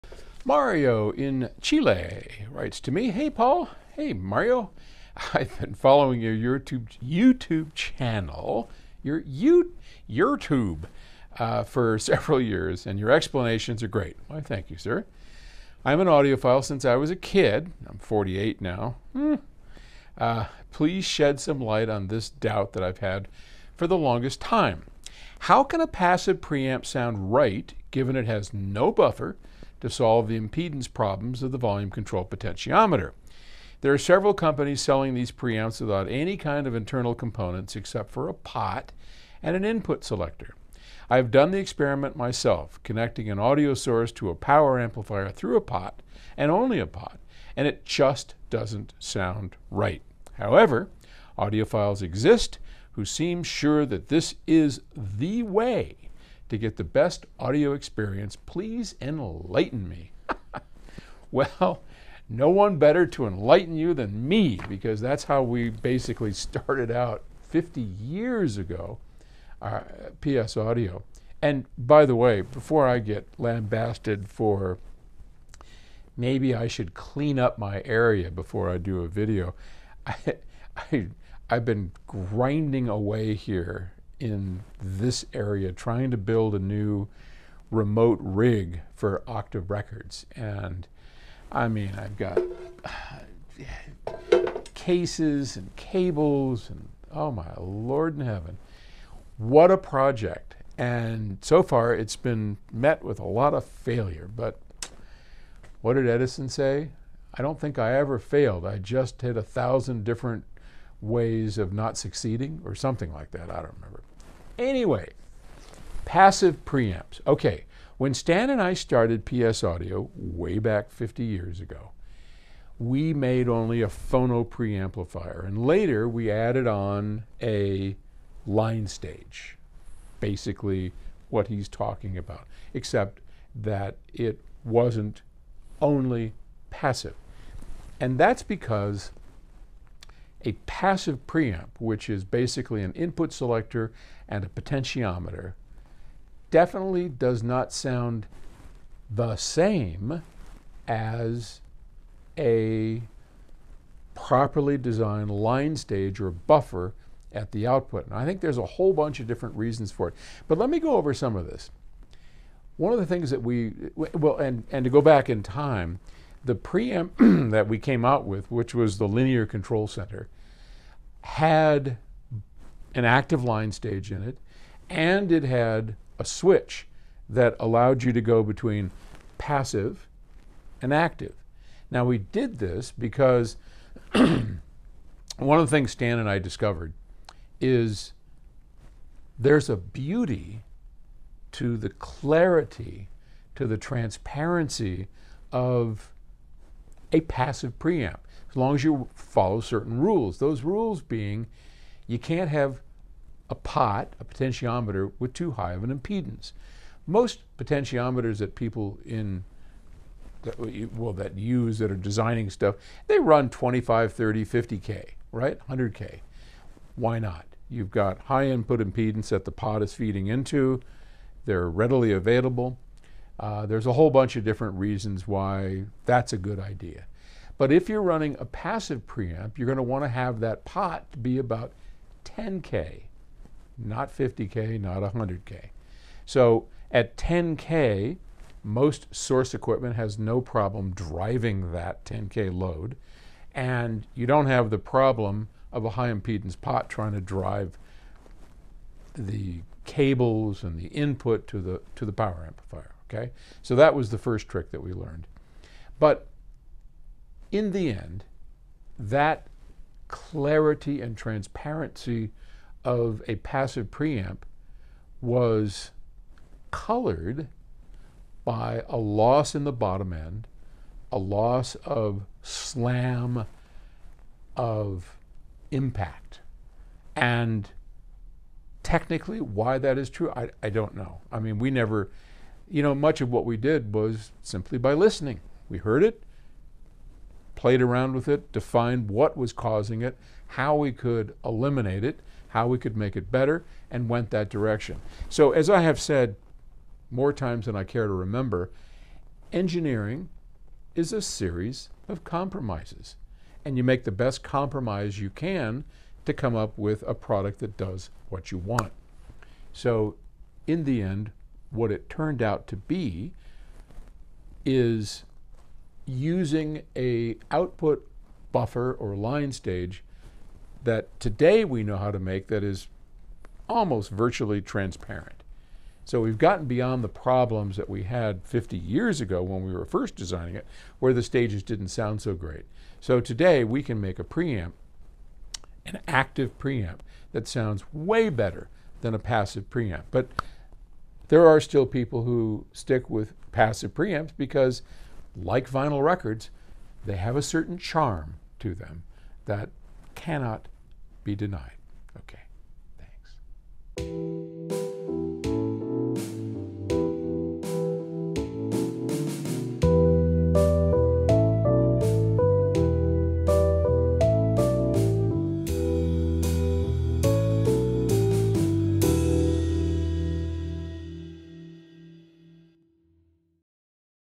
Passive Preamplifiers
Passive preamplifiers often get a bad rap. Is it accurate?